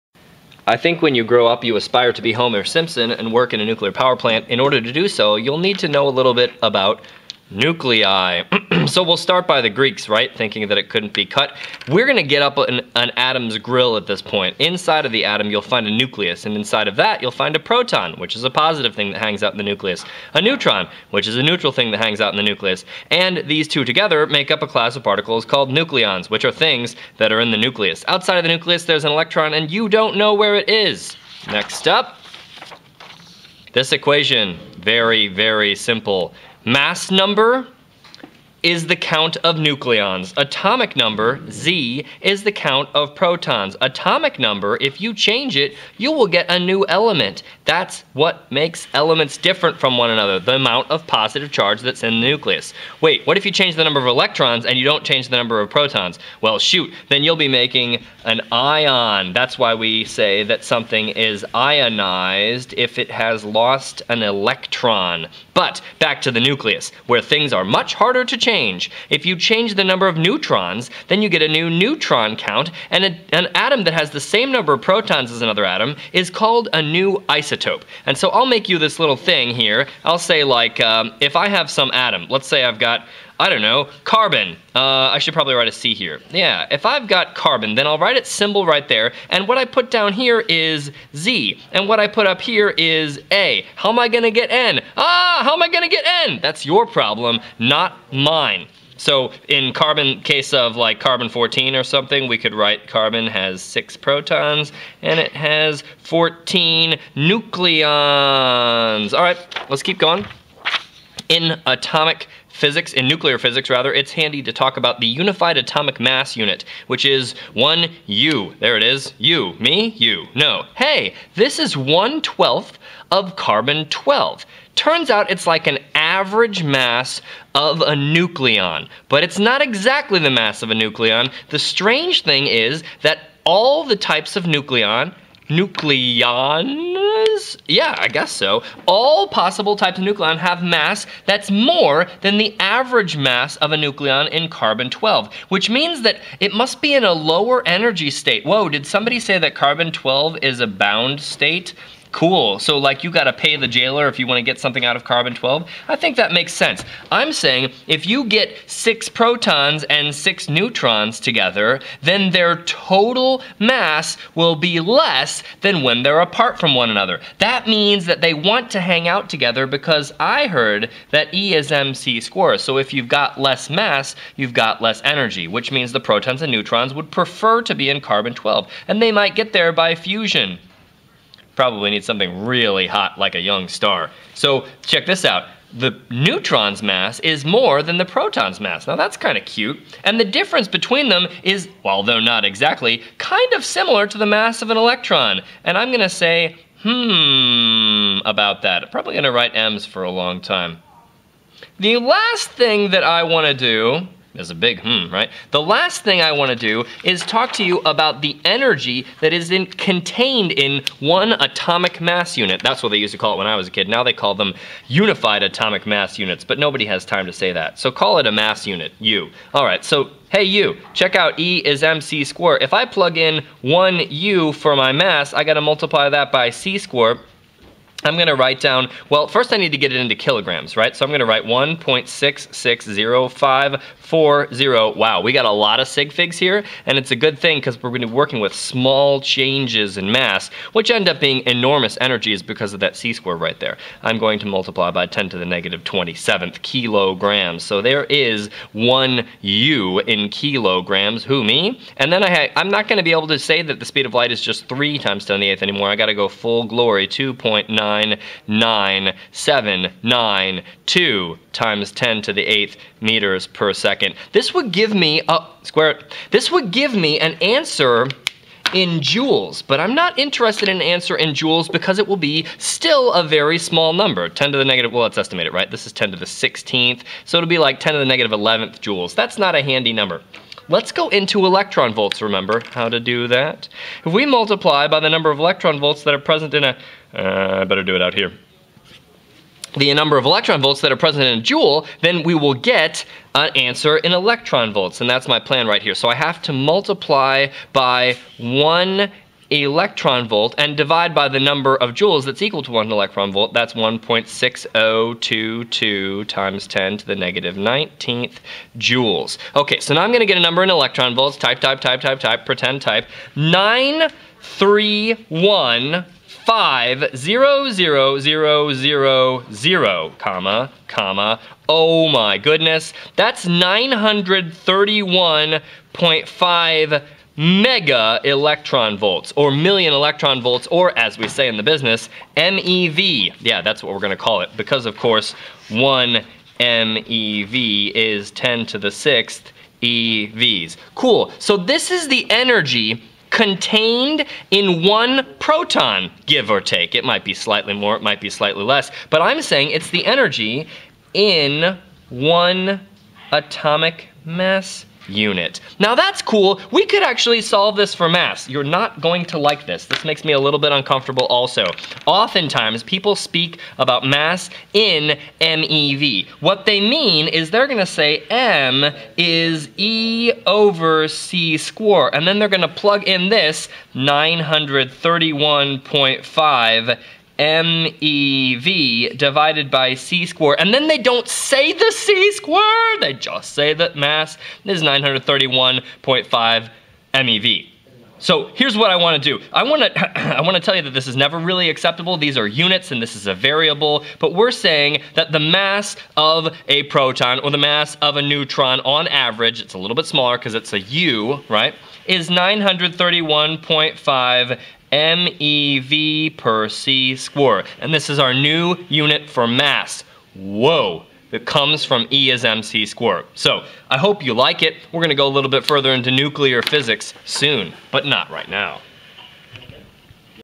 Intro to Nuclear Physics | Doc Physics
We'll see how mass can be measured in really funky units and why carbon 12 weighs less than the sum of its constituent parts.